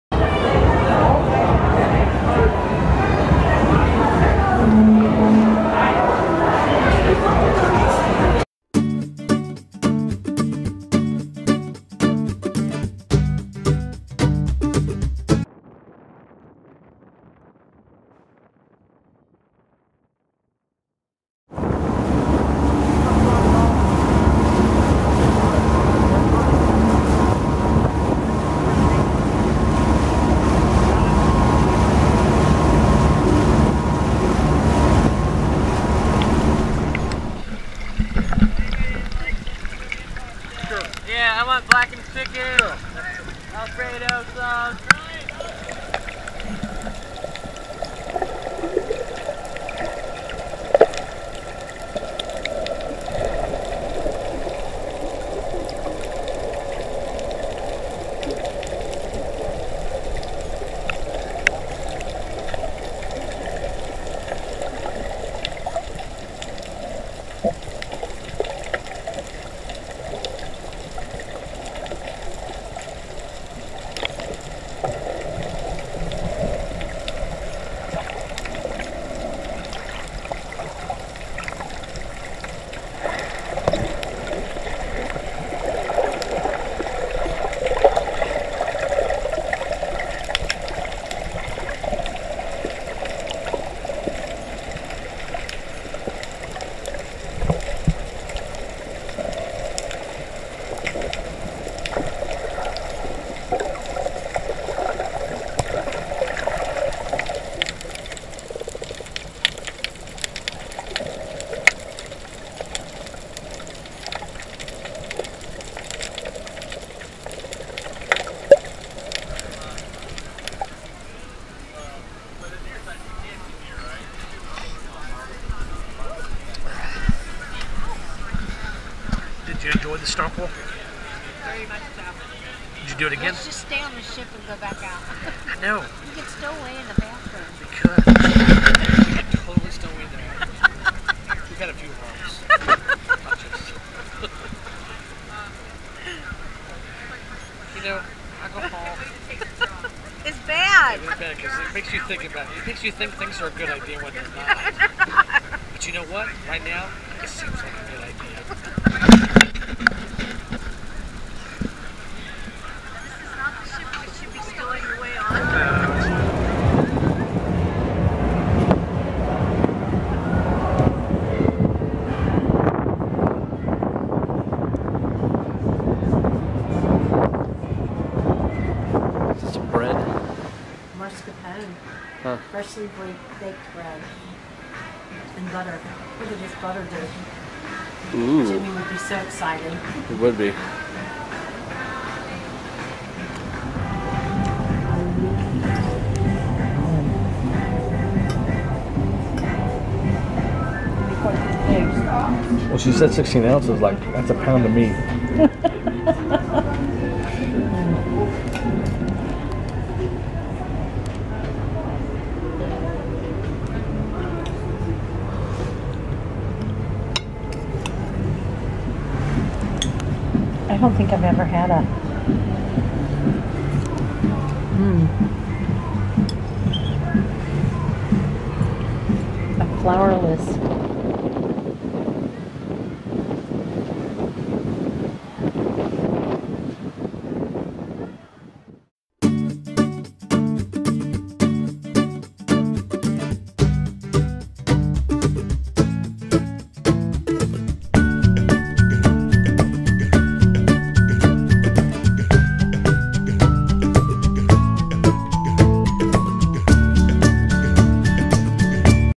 Cruise'n Day 5 04-15-2015 day 135
Day 5 of our Norwegian Getaway Cruise we visit St. Thomas

Featured videos

Heath